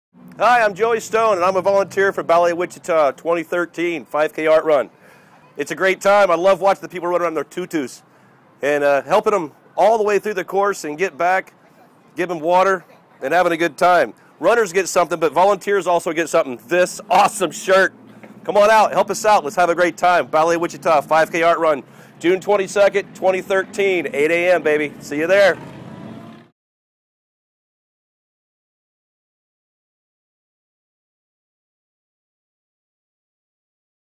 Joey Stone - Race Volunteer - Ballet Wichita 5K Art Run
Awesome volunteer Joey Stone with a personal invitation to RUN, PAINT and PARTY at the 2nd Annual Ballet Wichita 5K Art, June 22, 2013 in Wichita, Kansas.